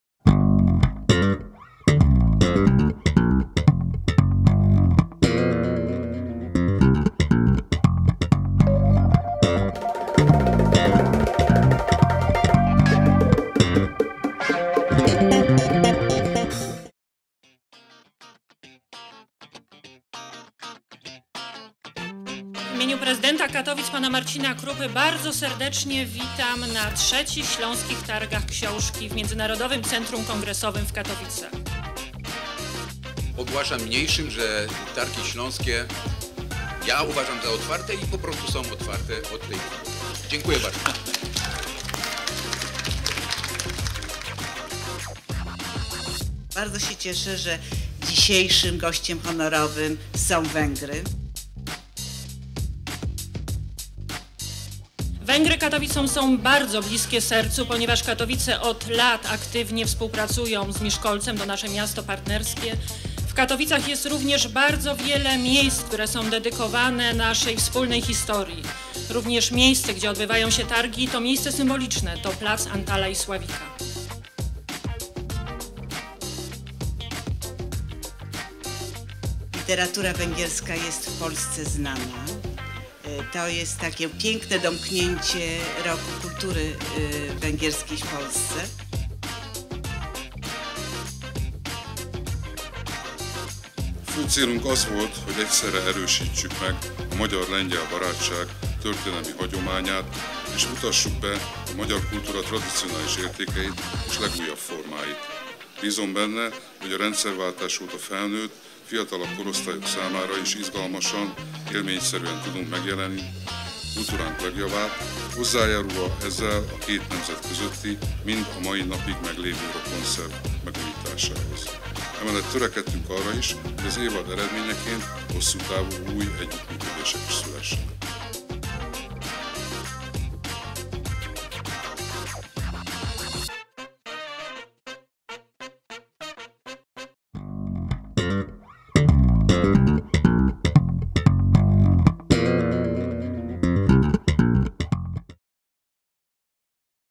Otwarcie Śląskich Targów Książki w Katowicach
Rok Kultury Węgierskiej 2016/2017 zaowocował wydaniem ponad 50 madziarskich tytułów tłumaczonych na język polski, poruszających tematykę węgierską. Śląskie Targi Książki, gdzie Węgry były Gościem Honorowym, stanowiły doskonałą okazję, aby zapoznać się z literackim dorobkiem tych 18 miesięcy oraz wziąć udział w spotkaniach autorskich. Targi trwały od 10 do 12 listopada w Międzynarodowym Centrum Kongresowym w Katowicach.